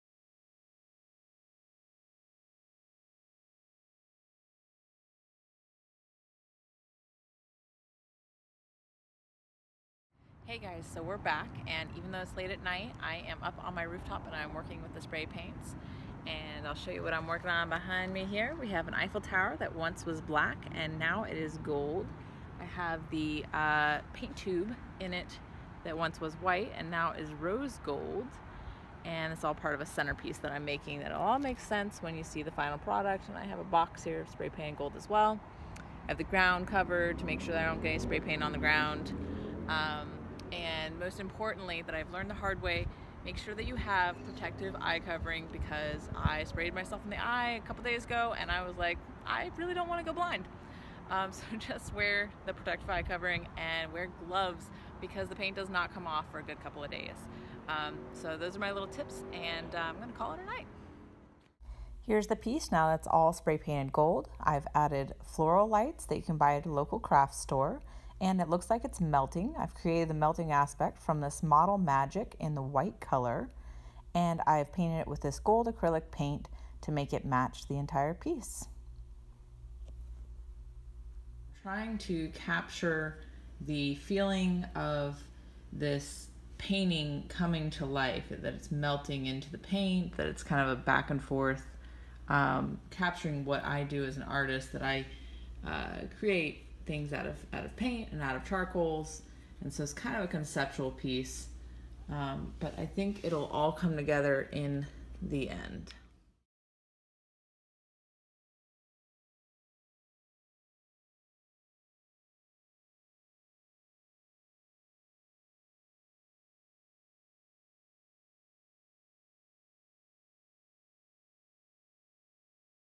Creating a Centerpiece for the Maitland Art Center Art Gala Participation
How I created a melting Eiffel Tower for the Art Gala called Participation at the Maitland Art Center